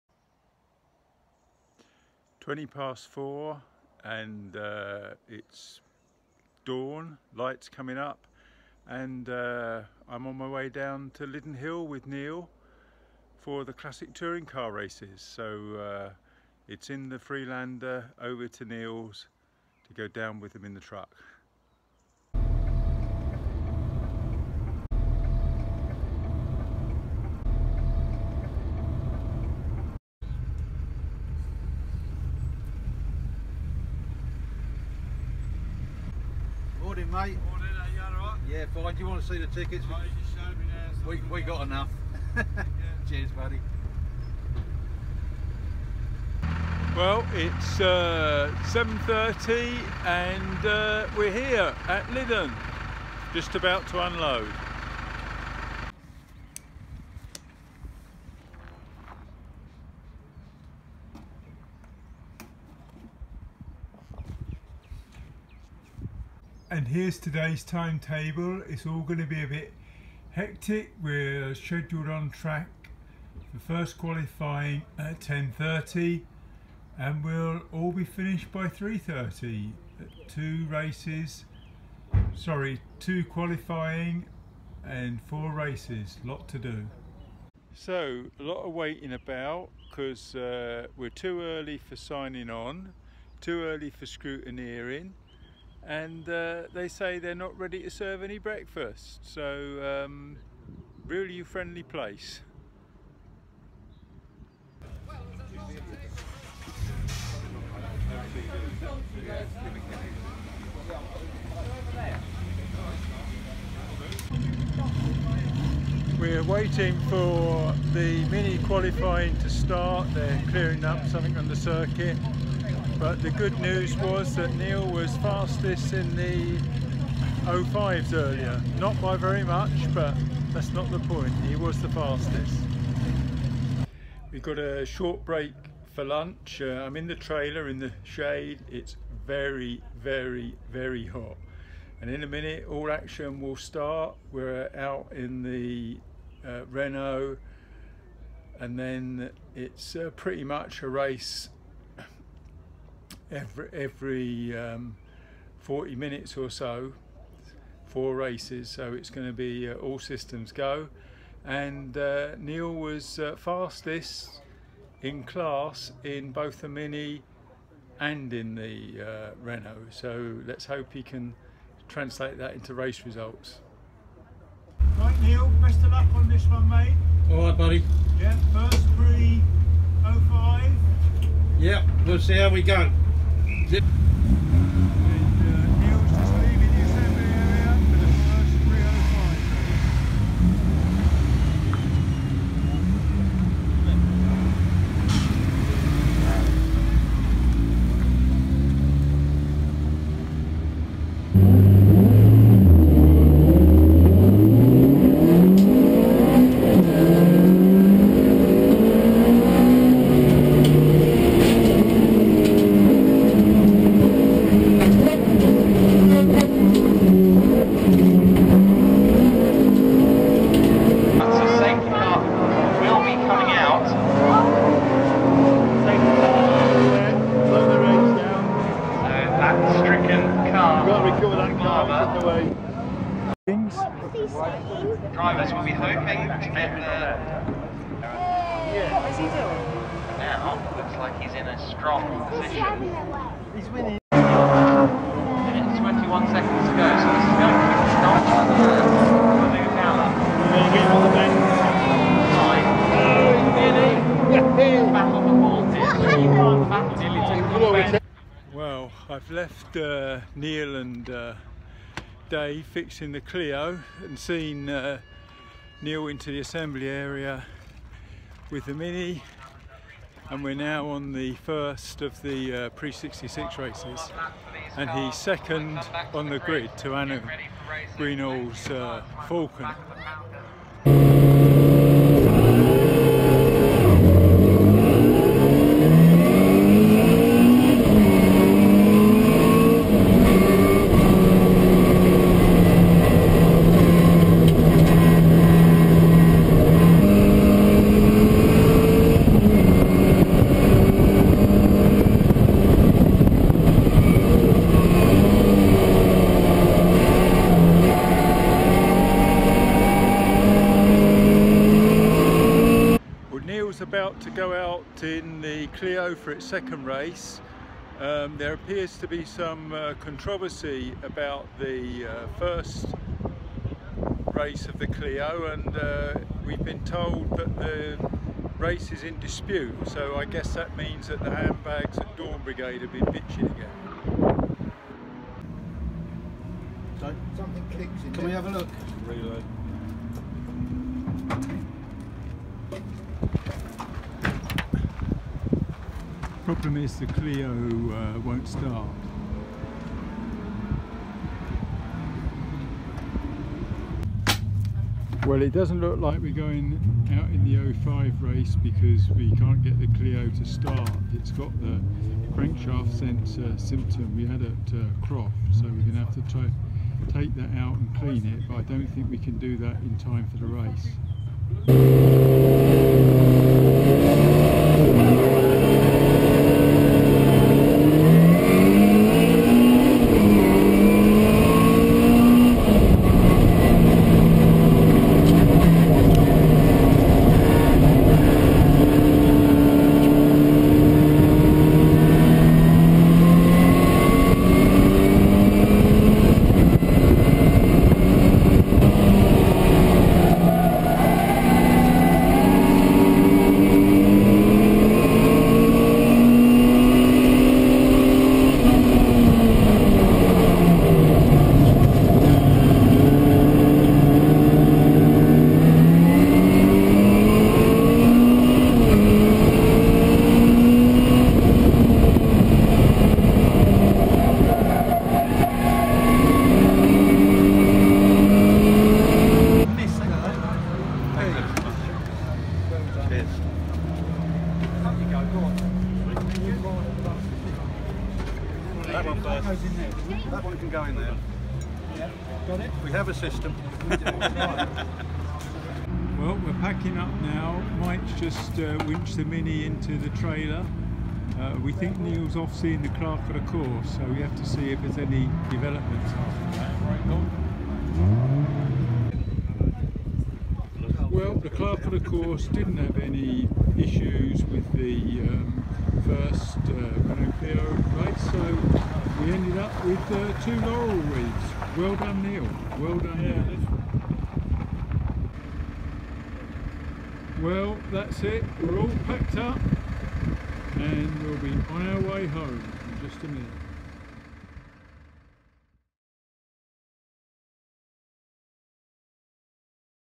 2018 07 Lydden VBlog
My day at Lydden Hill on 8 July 2018.